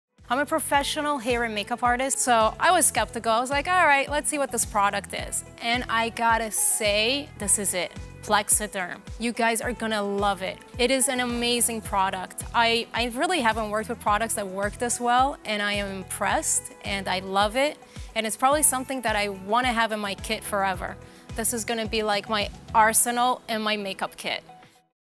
Plexaderm - The Arsenal In My Makeup Kit
Professional make-up artist works with Plexaderm, and is quickly impressed by how well Plexaderm reduces eye bags and wrinkles. Watch Plexaderm visibly tighten under eye bags.